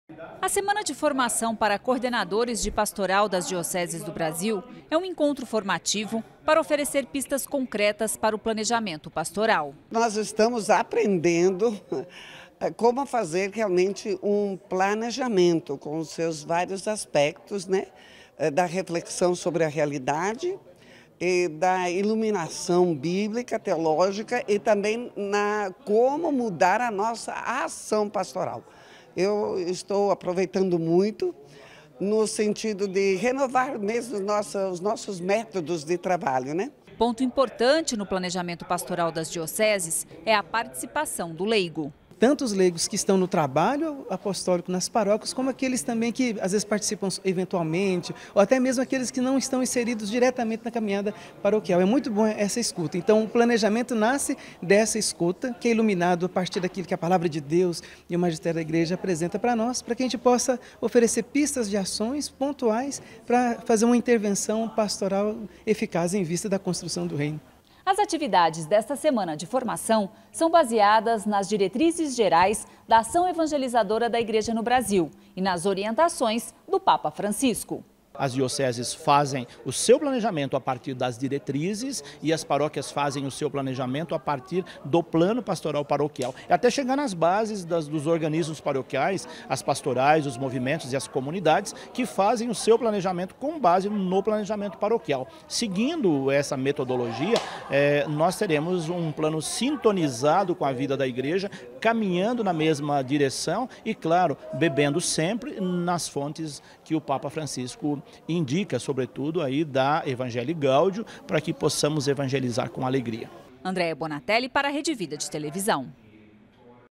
Encontro em Brasília aborda metodologia e planejamento pastoral - JCTV - 03/05/18
Coordenadores de pastoral participam de semana de formação em Brasília. O tema do encontro é Metodologia e Planejamento Pastoral. A reportagem é com Andréa Bonatelli.